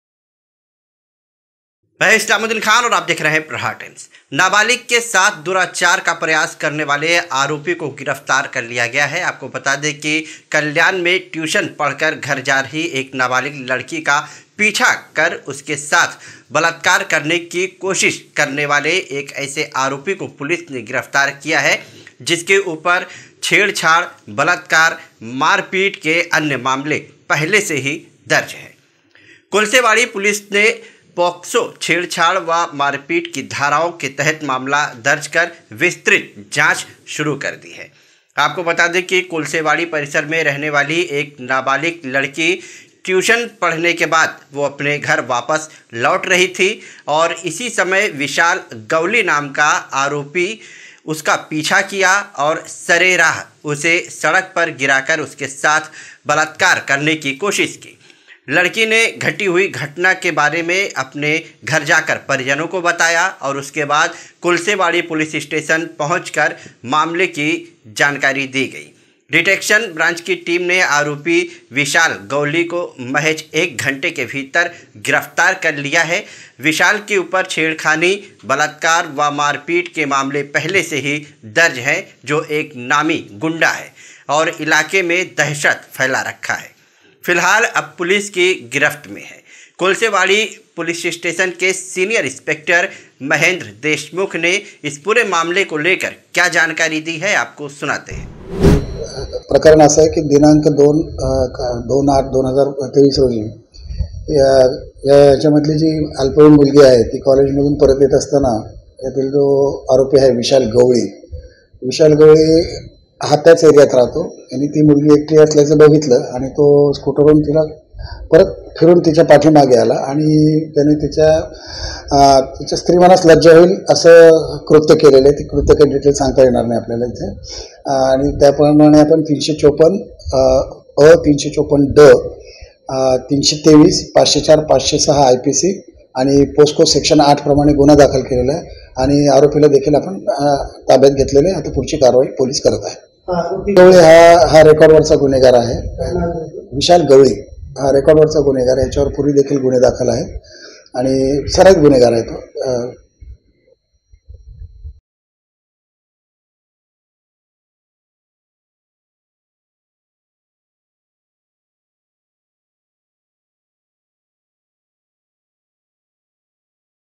ट्यूशन से घर लौट रही लड़की के साथ रेप करने की कोशिश,आरोपी गिरफ्तार || Islamuddin Khan | Prahar Times
ट्यूशन से घर लौट रही लड़की के साथ रेप करने की कोशिश,आरोपी गिरफ्तार | Islamuddin Khan

नाबालिग के साथ दुराचार का प्रयास करने वाले आरोपी को किया गिरफ्तार किया है,,कल्याण में ट्यूशन पढ़कर घर जा रही एक नाबालिग लड़की का पीछा कर उसके साथ बलात्कार करने की कोशिश करने वाले एक ऐसे आरोपी को पुलिस ने गिरफ्तार किया है जिसके ऊपर छेड़छाड़, बलात्कार,  मारपीट के अन्य मामले पहले से ही दर्ज हैं। कोलसेवाड़ी पुलिस ने पोक्सो, छेड़छाड़ व मारपीट की धाराओं के तहत मामला दर्ज कर विस्तृत जांच शुरू की है।,,कोलसेवाड़ी परिसर में रहने वाली एक नाबालिग लड़की ट्यूशन पढ़ने के बाद अपने घर वापस जा रही थी। इसी समय विशाल गवली नामक आरोपी ने उसका पीछा किया और सरेराह उसे सड़क पर गिराकर उसके साथ बलात्कार की कोशिश की। लड़की ने घटी हुई घटना को अपने परिजनों को बताया और कोलसेवाड़ी पुलिस स्टेशन पहुंचे। मामले की जानकारी मिलते ही डिटेक्शन टीम ने आरोपी विशाल गवली को महज एक घंटे के भीतर गिरफ्तार कर लिया। विशाल के ऊपर छेड़छाड़, बलात्कार व मारपीट के मामले पहले से ही दर्ज हैं जो एक नामी गुंडा है और इलाके में दहशत फैला रखा है। फिलहाल अब पुलिस की गिरफ्त में है,,,कोलसेवाड़ी के सीनियर इंस्पेक्टर महेंद्र देशमुख ने क्या जानकारी दी है